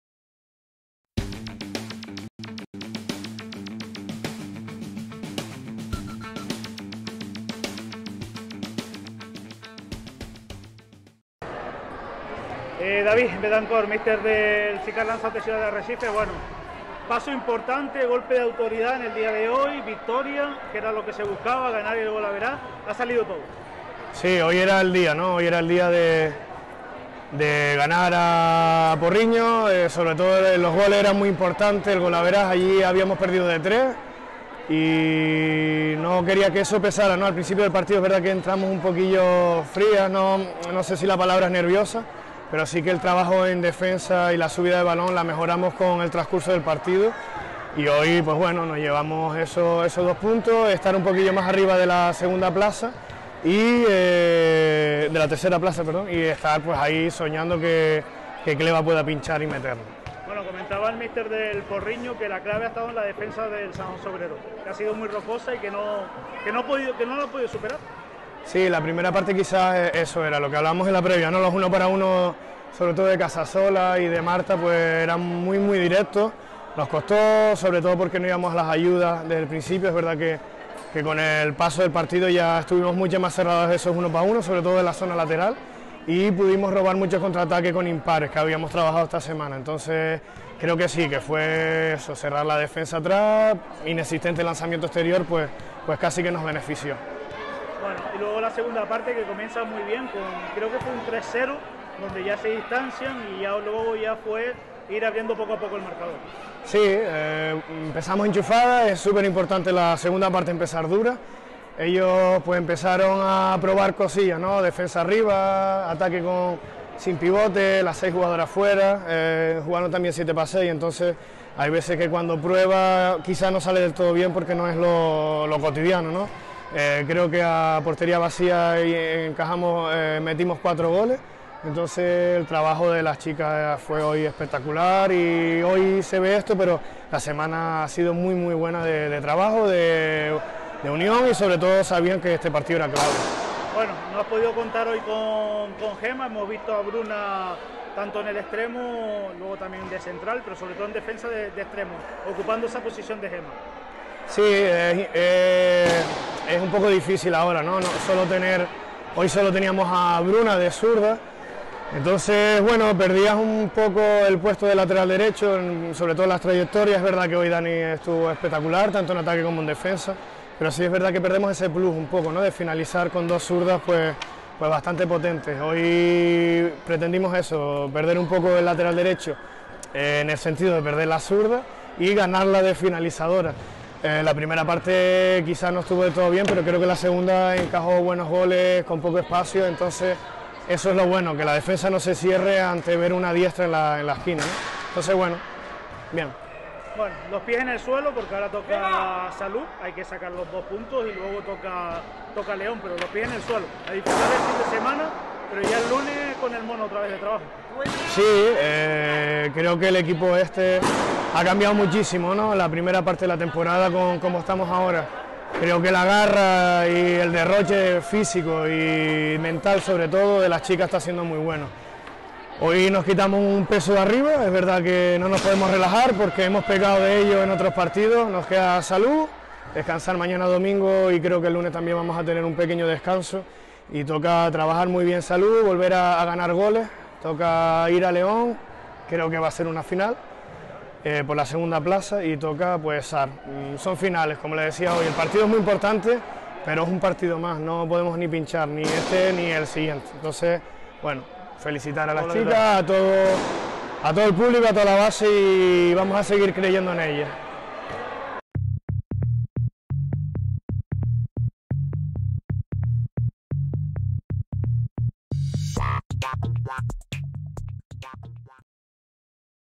David Betancort. San José Obrero vs BM Porriño
Entrevista al entrenador del CICAR Lanzarote Ciudad de Arrecife, David Betancort, a la finalización del encuentro disputado ante el Rubensa Balonmano Porriño, correspondiente a la 23º jornada de la División de Honor Plata Femenina.